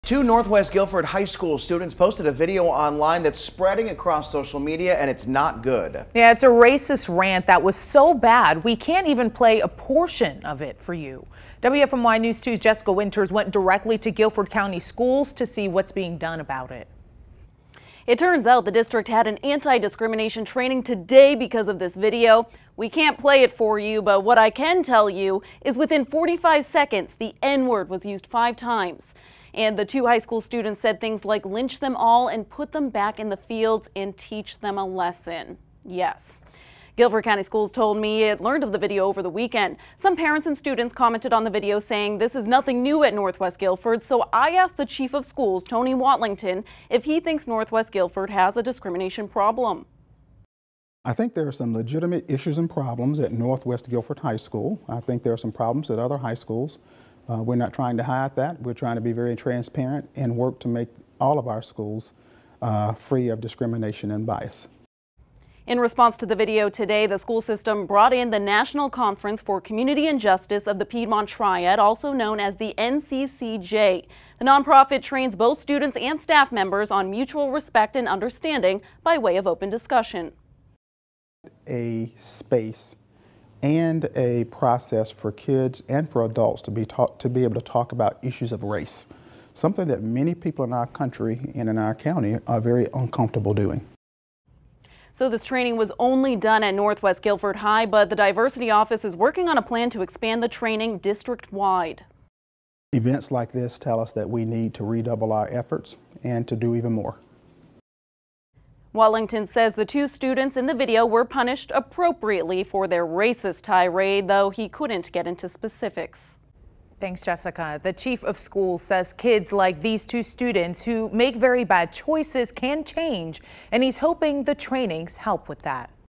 Racist Rant Caught On Camera Leads Guilford County Schools To Take Action
After students were identified in a racist video online, GCS brought in the National Conference for Community and Justice to train students and staff about respect and understanding.